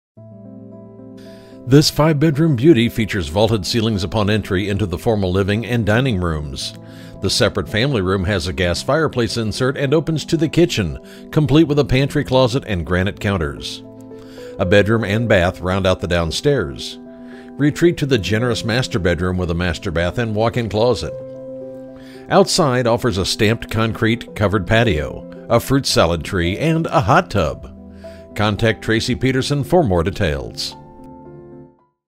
8424 Felton Crest Way Elk Grove, CA | ColdwellBankerHomes.com
8424 Felton Crest Way, Elk Grove, CA

This five bedroom beauty features vaulted ceilings upon entry into the formal living and dining rooms. A spacious bedroom and a full bath downstairs could easily be a second master bedroom or guest quarters. The separate family room has a gas fireplace insert and stunning wood laminate flooring that